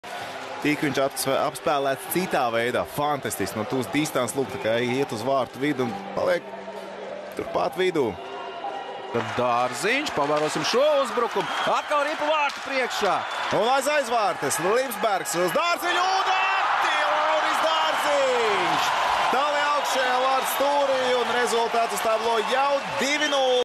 Dinamo Riga 2 - 1 Kunlun Red Star February 24, 2020
(0:1) Carter Ashton (Lauris Dārziņš, Miķelis Rēdlihs)
(0:2) Lauris Dārziņš (Jānis Švānenbergs, Roberts Lipsbergs)
(1:2) Devante Smith-Pelly (Ryan Sproul, Victor Bartley)

Kunlun Red Star Goalie: Jeremy Smith
Dinamo Riga Goalie: Alexander Salak